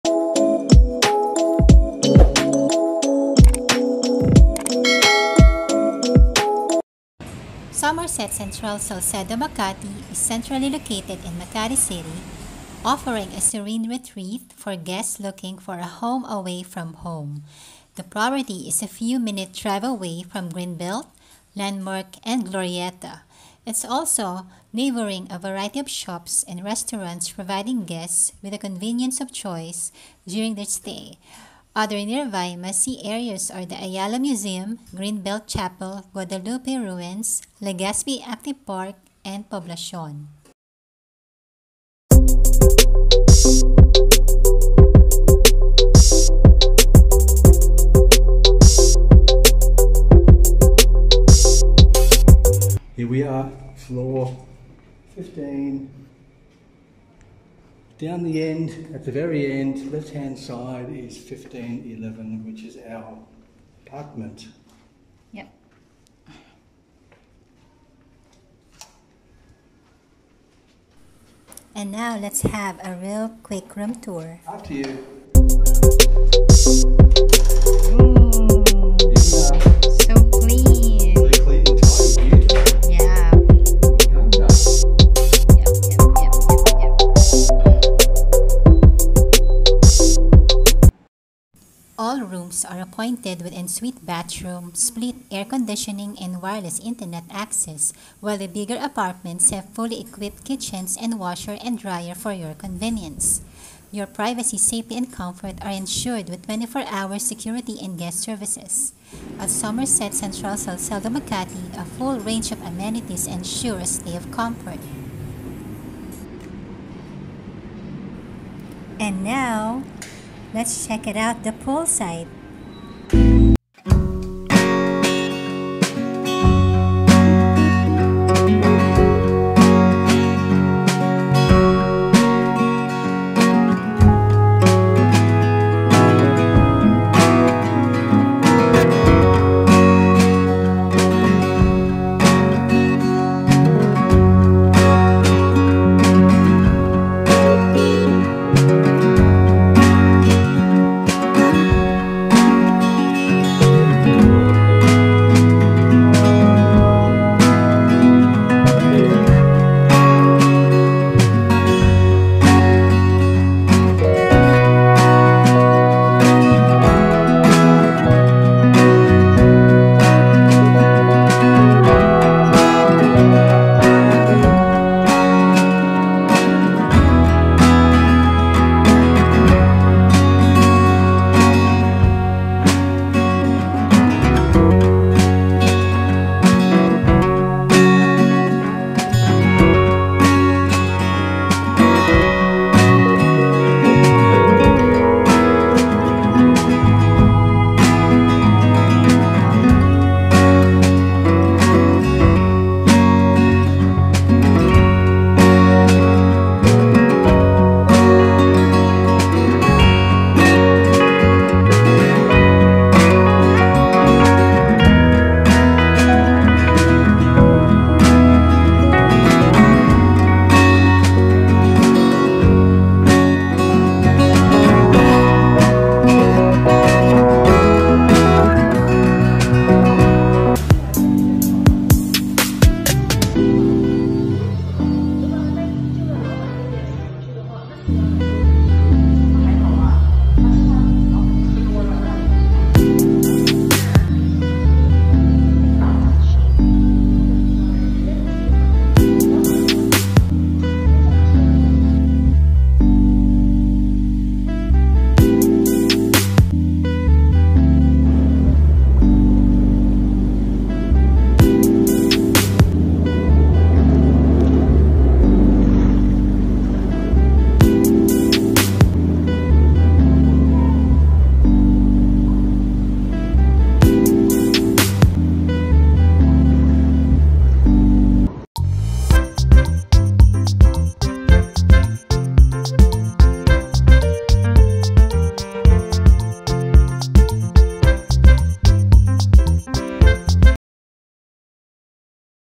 SOMERSET CENTRAL SALCEDO MAKATI I STAYCATION SERVICED APARTMENT TOUR
Serviced Apartment in Makati
Somerset Central Salcedo Makati is centrally located in Makati City offering a serene retreat for guests looking for a 'home away from home'. The property is a few-minute drive away from Greenbelt, Landmark and Glorietta. It's also neighbouring a variety of shops and restaurants providing guests with the convenience of choice during their stay. Other nearby must-see areas are the Ayala Museum, Greenbelt Chapel, Guadalupe Ruins, Legazpi Active Park, and Poblacion.

The 285-room property offers five types of units ranging from Studio, Studio Premier, One-Bedroom Deluxe, Two-Bedroom Deluxe and Three-Bedroom Deluxe units. All rooms are appointed with ensuite bathroom, split air-conditioning and wireless internet access while the bigger apartments have fully-equipped kitchens and washer and dryer for your convenience. Your privacy, safety and comfort are ensured with 24-hour security and guest services. At Somerset Central Salcedo Makati, a full range of amenities ensure a stay of comfort.

Wind down at the end of the day at the property's pool area, invigorate your healthy lifestyle in our fully-equipped fitness centre while engaging with our personal fitness trainers, and savour authentic meals and sip cocktails with the skyline view of the city. Enjoy the convenience of state-of-the-art audiovisual systems in our event spaces to cater to your needs whether its socials or meetings.

Choose from an array of favourites in our flexible menu to suit your taste. Somerset Central Salcedo Makati offers daily breakfast, housekeeping service, and grocery shopping service to help complement your lifestyle.

Amenities at Somerset Central Salcedo Makati
Somerset Central Salcedo Makati offers the perfect choice for contemporary accommodation in Makati. Guests have an array of amenities at their fingertips to help them live like they're in their own home.

Enjoy the harmony of city life with the exceptional services in the property. Explore the delights of the city while appreciating the modern amenities of your home away from home.

Residence Features & Amenities
En-suite bathroom with towels & toiletries
Fully-equipped kitchen
Hair dryer
Home entertainment system
Non-smoking apartments
Steam iron & ironing board
Telephone (with IDD) *
Telephone with IDD & voicemail facilities *
Rest & Relax
Children’s playroom
Fully-equipped gymnasium
Residents' programmes
Spa *
Swimming pool

Comfort & Convenience
24-hour reception
Airport transfer service *
Breakfast lounge
Daily breakfast
Carpark *
Doctor-on-call *
F&B lounge *
Daily housekeeping service
Daily housekeeping service (except Sundays & public holidays)
Launderette *
Limousine service *
Room service *
Shuttle bus services *
Business Matters
Broadband internet access
Business centre services *
Business corner
Conference rooms *
Courier services *
Internet corner
Secretarial services *
Wireless internet connectivity (WiFi)